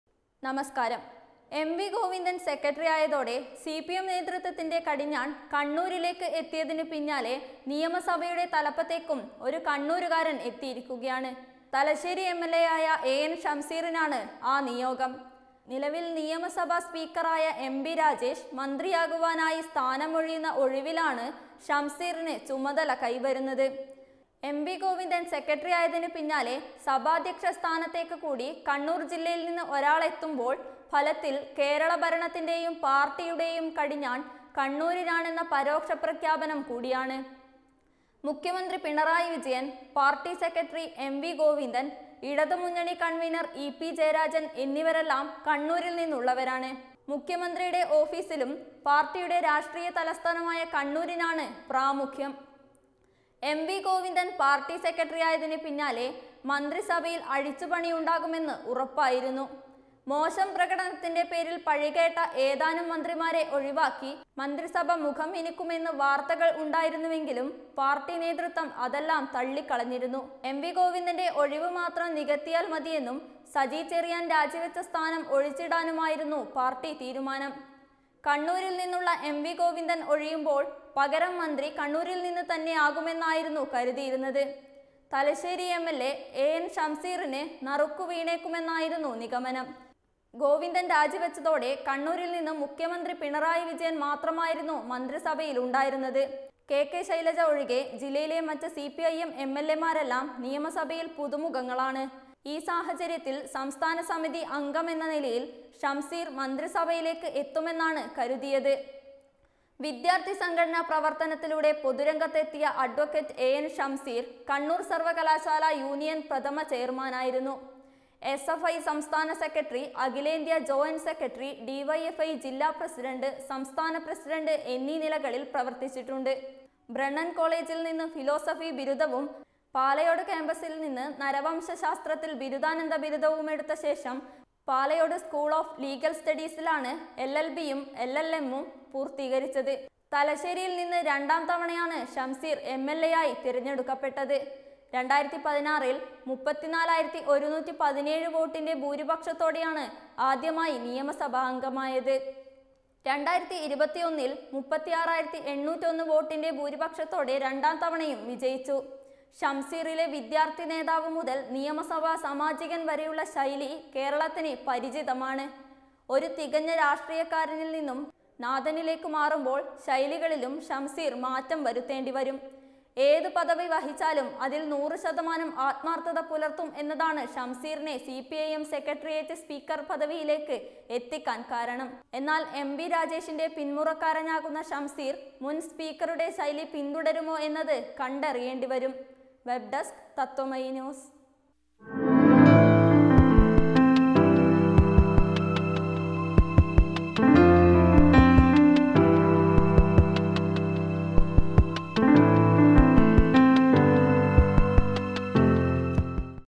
മന്ത്രിസഭയിലെ അഴിച്ചുപണിക്ക് പിന്നിലെ രഹസ്യങ്ങൾ ഇങ്ങനെ I KANNUR LOBBY
മുഖ്യമന്ത്രിയും സ്പീക്കറും പാർട്ടി സെക്രട്ടറിയും എല്ലാം കണ്ണൂർ ലോബിയുടെ ഭാഗം